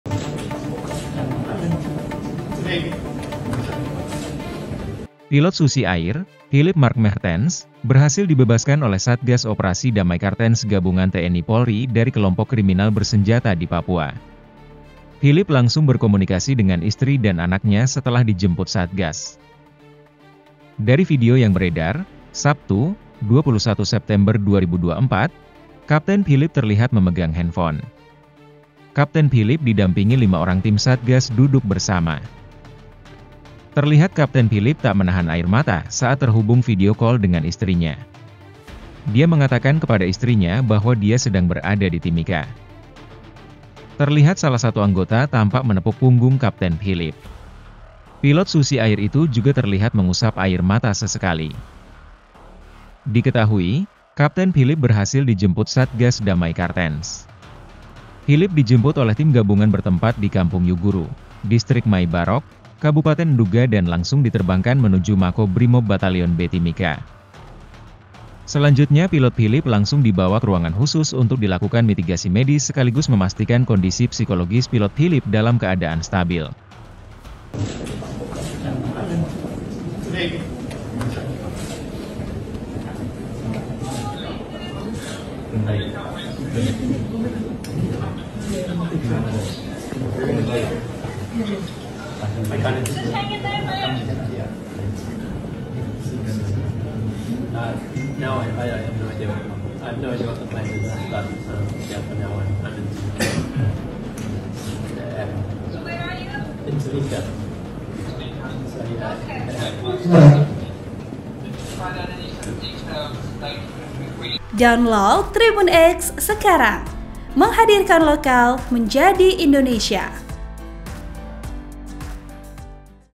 Momen Haru Pilot Susi Air Video Call Keluarga usai Dibebaskan dari KKB
POS-KUPANG.COM - Pilot Susi Air, Philip Mark Mehrtens, berhasil dibebaskan oleh Satgas Operasi Damai Cartenz gabungan TNI-Polri dari kelompok kriminal bersenjata (KKB) di Papua. Philip langsung berkomunikasi dengan istri dan anaknya setelah dijemput Satgas.

Dari video yang beredar, Sabtu (21/9/2024), Kapten Philip terlihat memegang handphone. Kapten Philip didampingi lima orang tim satgas duduk bersama.

Terlihat Kapten Philip tak menahan air mata saat terhubung video call dengan istrinya. Dia mengatakan kepada istrinya bahwa dia sedang berada di Timika.

Terlihat salah satu anggota tampak menepuk punggung Kapten Philip. Pilot Susi Air itu juga terlihat mengusap air mata sesekali.

Diketahui, Kapten Philip berhasil dijemput Satgas Damai Cartenz. Philip dijemput oleh tim gabungan bertempat di Kampung Yuguru, Distrik Maibarok, Kab Nduga dan langsung diterbangkan menuju Mako Brimob Batalyon B/Timika.

Selanjutnya Pilot Philip langsung dibawa ke ruangan khusus untuk dilakukan mitigasi medis sekaligus memastikan kondisi psikologis Pilot Philip dalam keadaan stabil.

Vp: Dhev